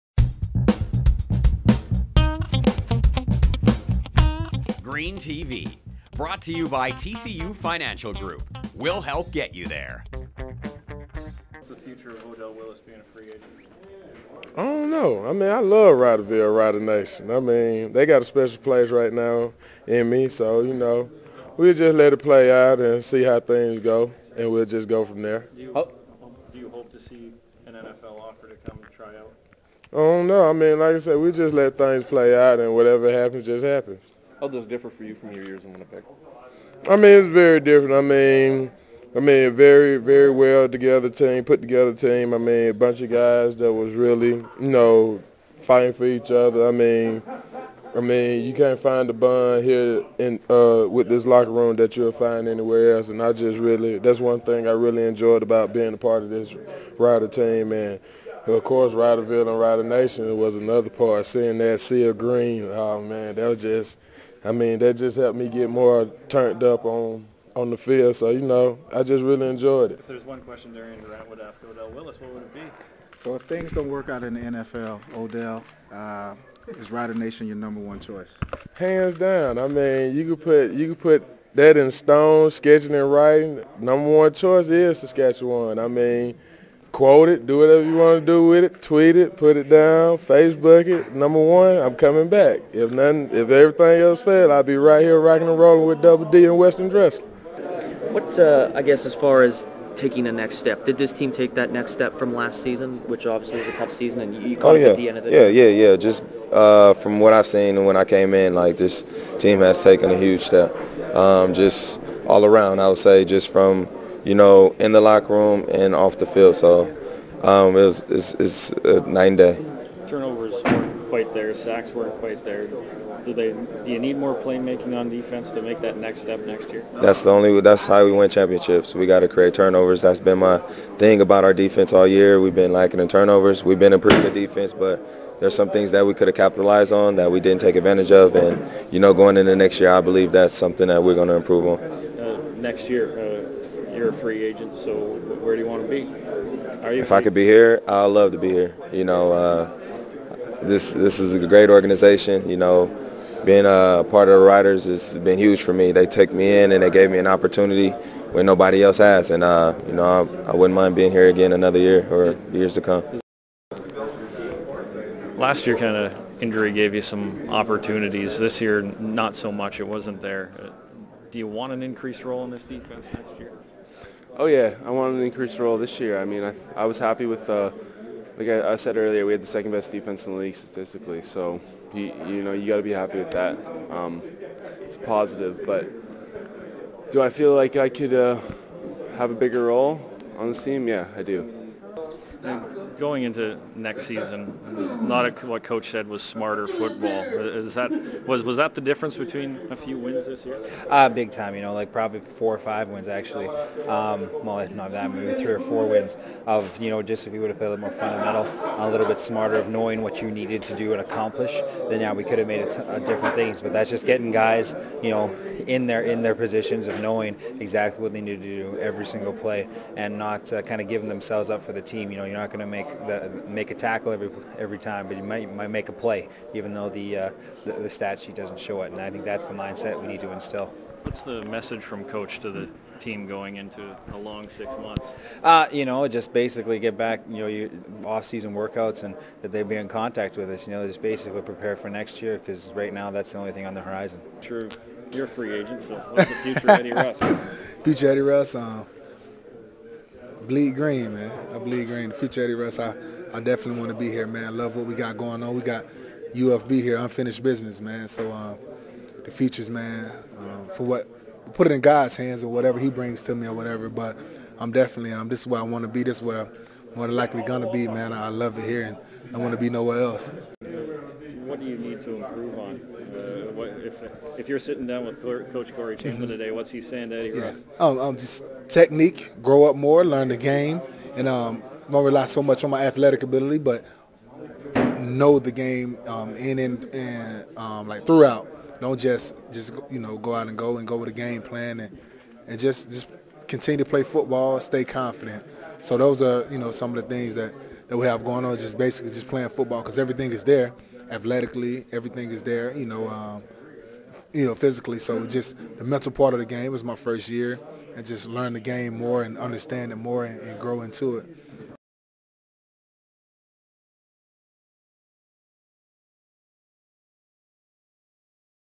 Green TV Garbage Bag Day - Defense
The Riders cleaned out their lockers for another year, Green TV met up with Odell Willis, Tyron Brackenridge, Craig Butler, Keith Shologan and Eddie Russ to talk about the year that was and the future.